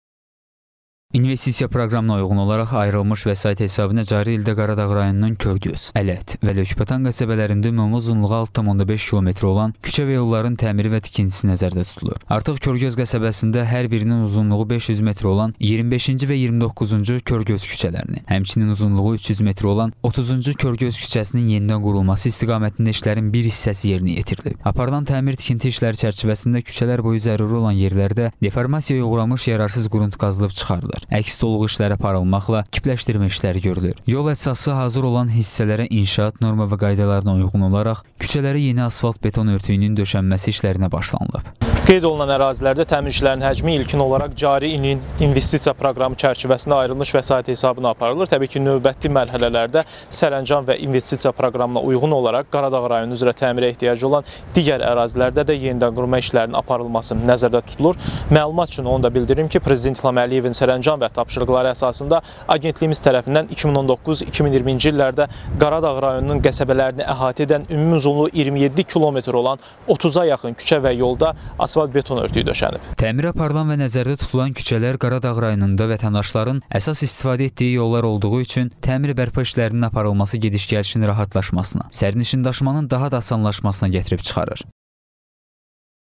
Qaradağda yollar təmir olunur
30-a yaxın küçə və yola asfalt-beton örtüyü döşənib